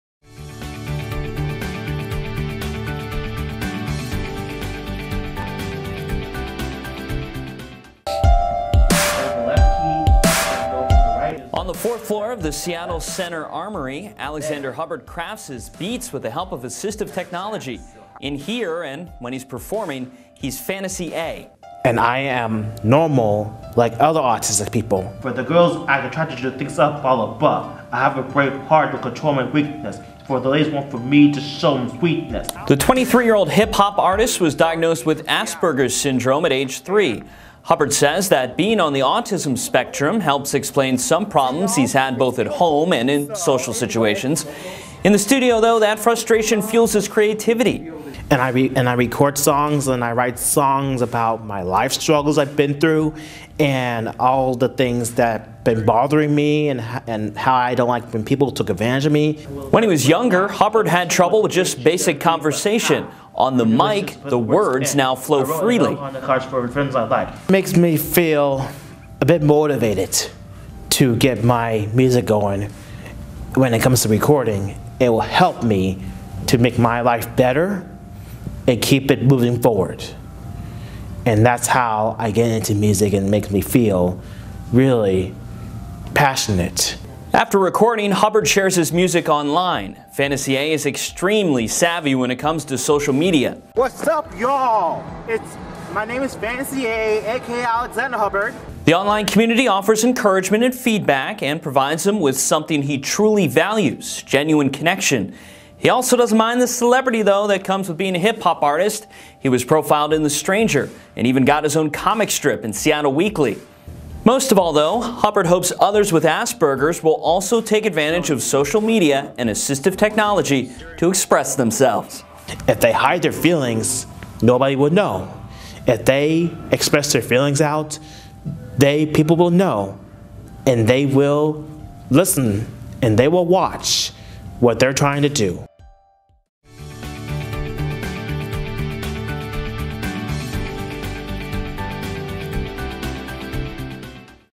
Assistive Technology - Alexander Hubbard
This video showcases the transformative effect Assistive Technology can have on people's lives. It was funded by Roads to Community Living, a Money Follows the Person federal grant from the U.S. Department of Health and Human Services. It is administered by the Washington State Department of Social and Health Services (DSHS).